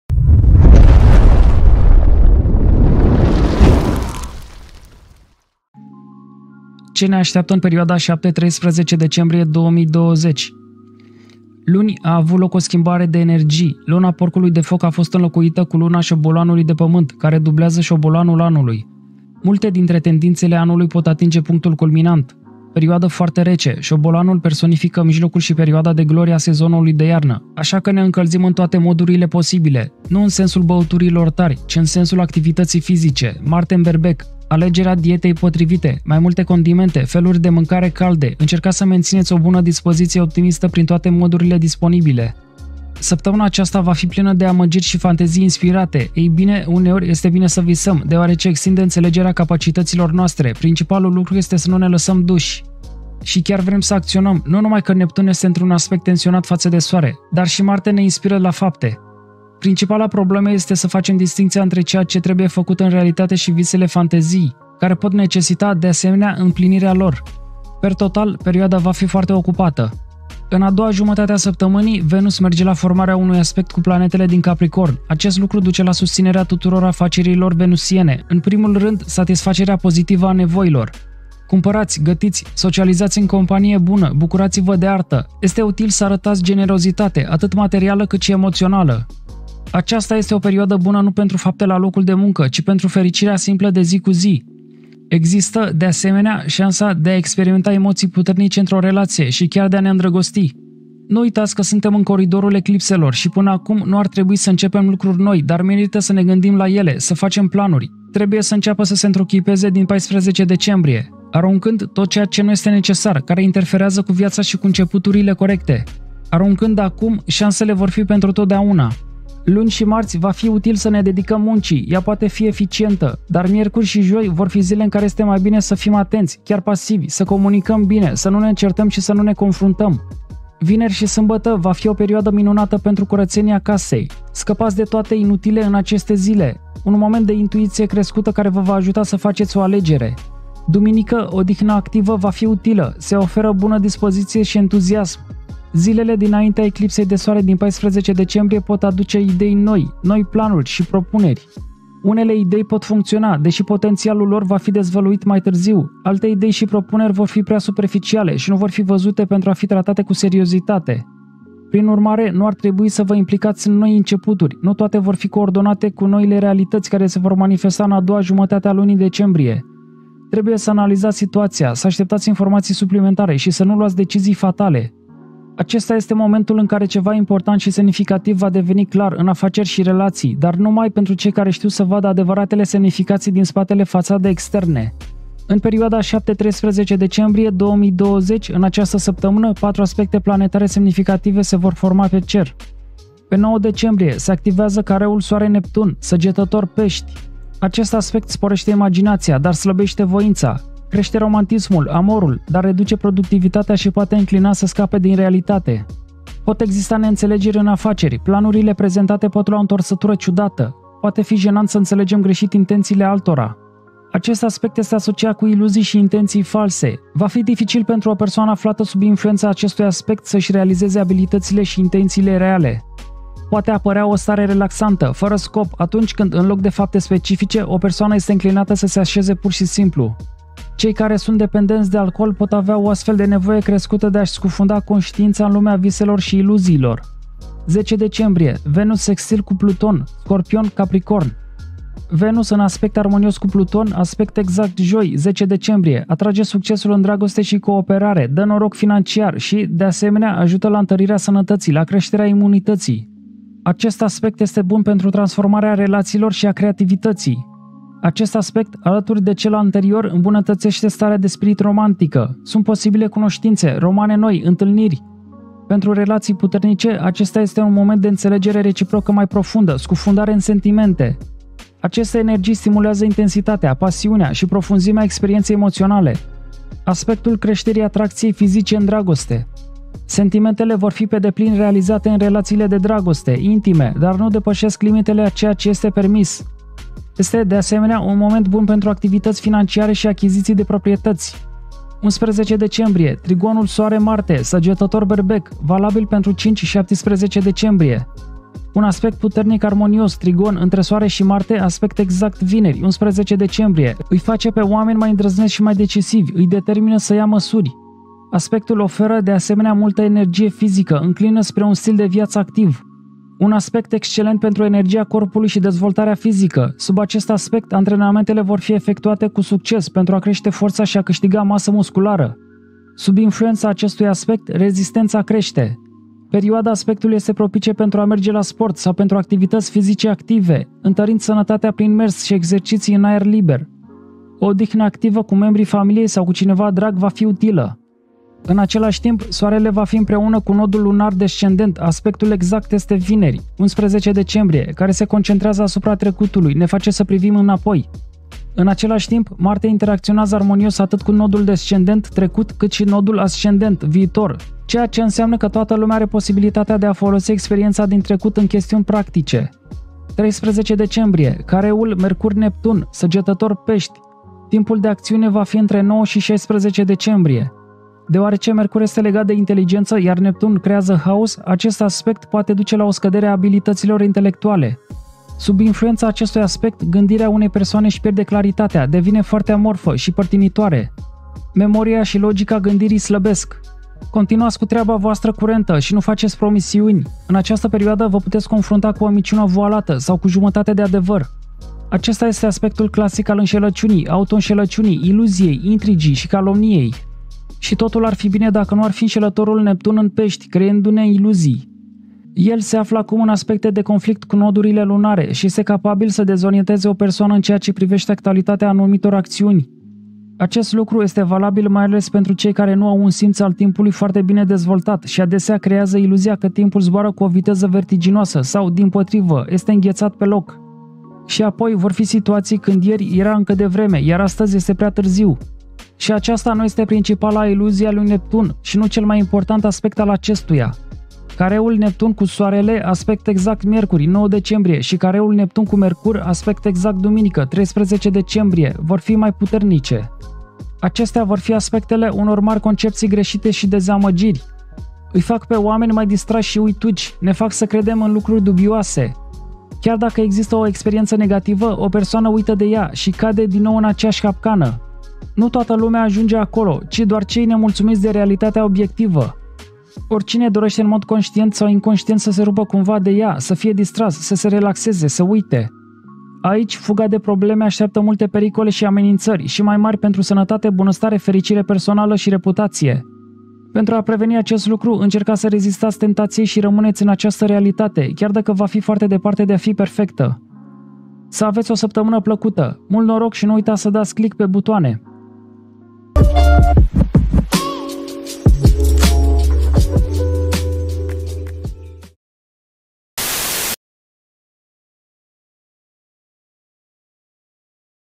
Ce ne așteaptă în perioada 7 - 13 Decembrie 2020
Săptămâna aceasta va fi plină de amăgiri și fantezii inspirate - ei bine, uneori este bine să visăm, deoarece extinde înțelegerea capacităților noastre, principalul lucru este să nu ne lăsăm duși. Și chiar vrem să acționăm - nu numai că Neptun este într-un aspect tensionat față de Soare, dar și Marte ne inspiră la fapte. Principala problemă este să facem distincția între ceea ce trebuie făcut în realitate și visele-fantezii, care pot necesita, de asemenea, împlinirea lor. Per total, perioada va fi foarte ocupată.